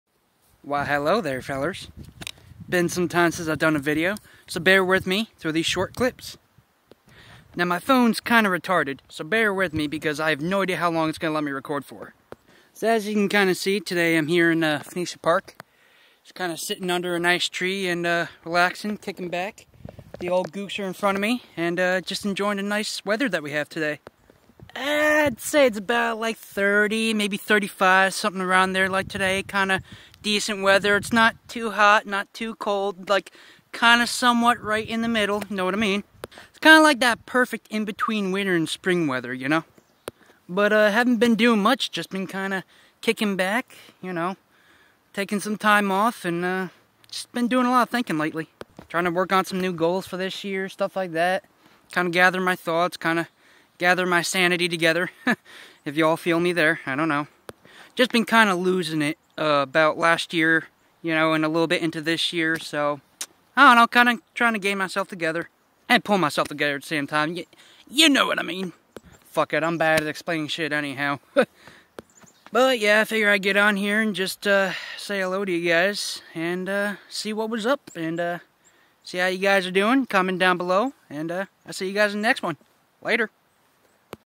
How ya fellers been doin?
Just wanted to get on here quick and check up on y'all. Also my phone's been giving me some problems lately with recording and storage space on this damn thing. So my videos will be short for now on until I get a new phone to record with.